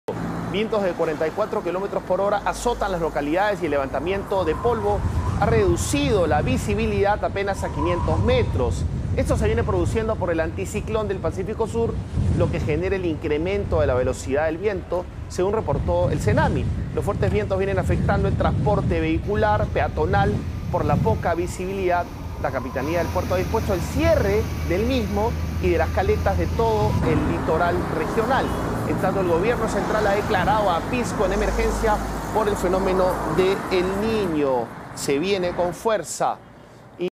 Pisco: Tormenta de polvo cubre la ciudad e impide la visibilidad en las calles
Automovilistas y transeúntes fueron sorprendidos por fenómeno ocasionado por la llegada de anticiclón.